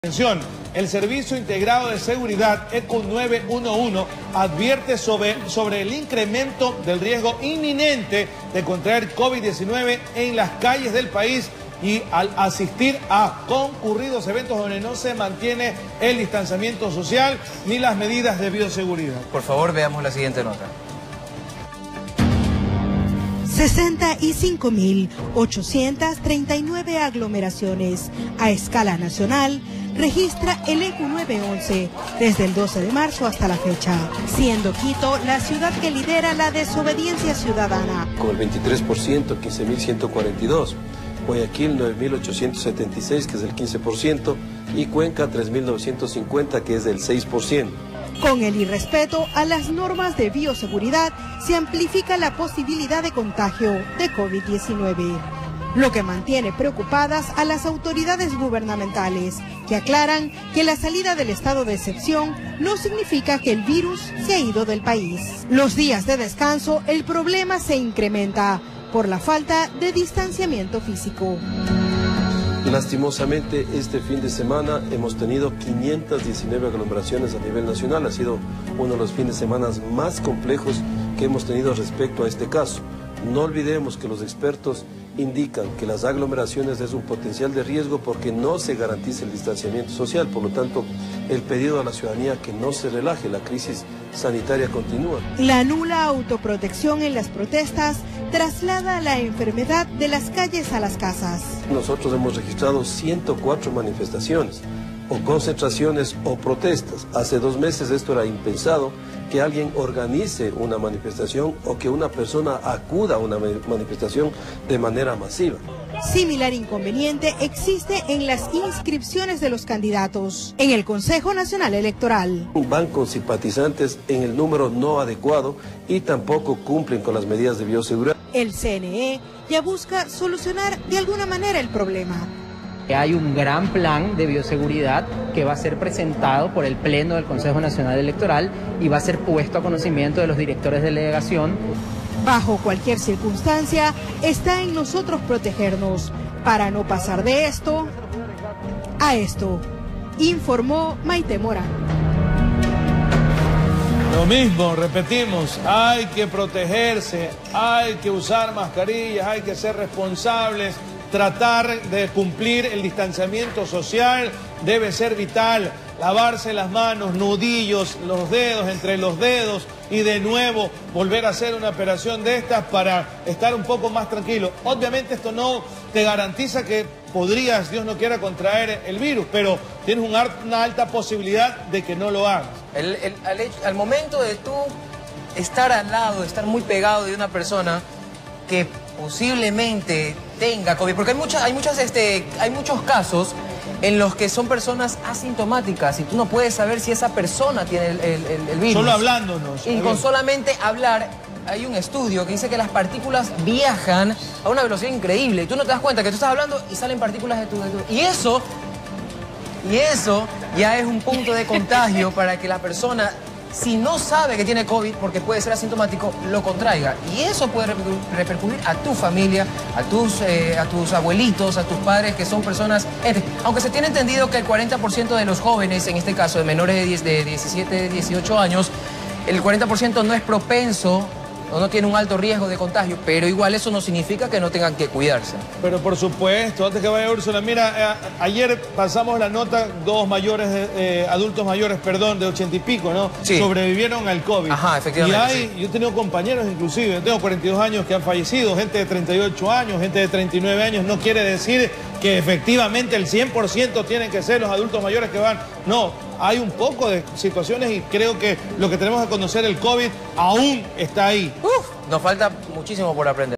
El ECU911 ha registrado 65.839 aglomeraciones de personas
Esta cifra corresponde a los últimos días y a escala nacional

Esta situación representa el riesgo inminente de contraer COVID-19, en las calles del país y al asistir a eventos concurridos.